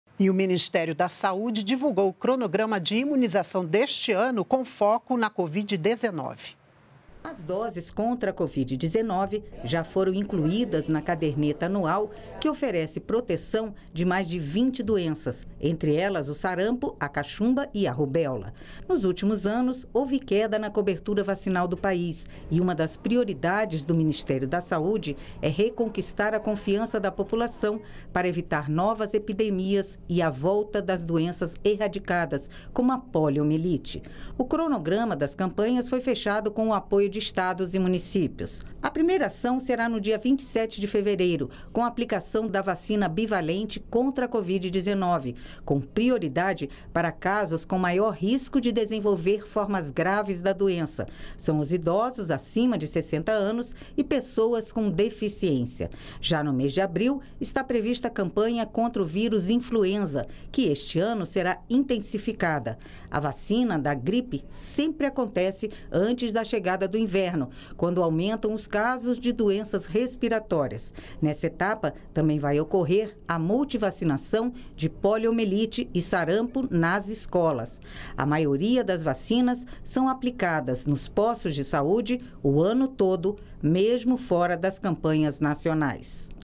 Saúde | Calendário de vacinação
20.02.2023 - O Ministério da Saúde divulgou o cronograma de vacinação deste ano, incluindo o imunizante bivalente contra a covid-19.